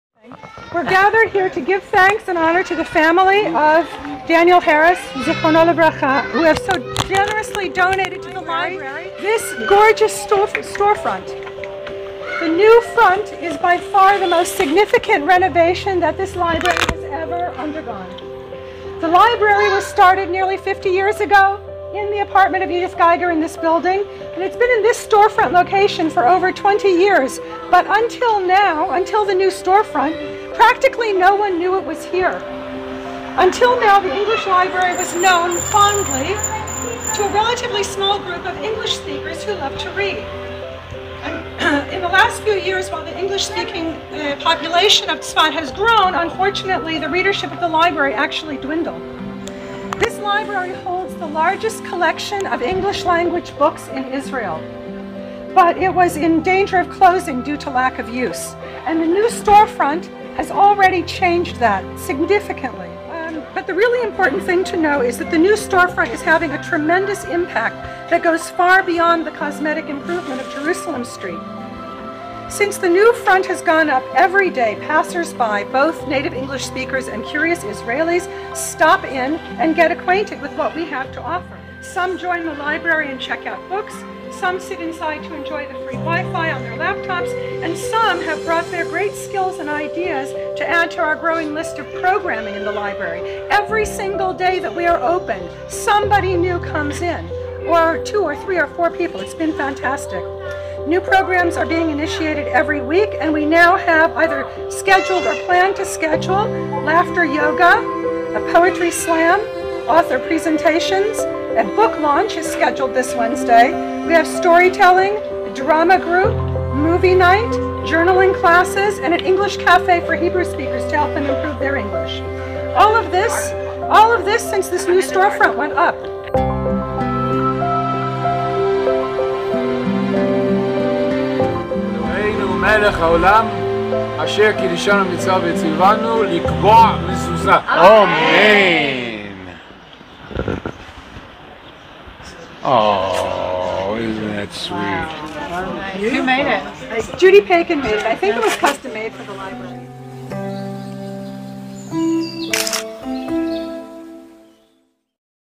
How many changes can you count at our renovated library?
The English Library is growing to meet the needs of our growing community:  "growing" forward, we're about books - and so much more!

'Empowering our community by supporting English language culture, literacy, and fluency."

With more space at our facility at 38 Jerusalem Street, Tzfat, devoted to our 15,000 books and community programming, we'll be holding classes on language, writing, hobbies and crafts:

Children's Library story-time, film screenings, author presentations, and holiday events are all planned for the coming year.

Our Garden Club resumes as the Shmitta year concludes, PLUS an exciting new permaculture initiative and children's garden program - with other surprises in store!

Additionally, we're going big online, with a renovated website and more active social media presence; we're planning meet-the-author podcasts, live and live-streamed events, increased community networking, and a focus on local events and alerts.

Join us growing forward in our newly-renovated spaces, using our online catalog, attending our film screenings, watching our podcasts, joining our literary and social programming, and taking part in our live and online events.

The English Library is here to support the literary and cultural initiatives of our community; we invite you to bring your suggestions and needs to us so that the library can help support your dreams!

Many thanks to our staff and volunteers, donors and supporters, and - of course - you the reader, viewer, and listeners of the Edyth Geiger Library.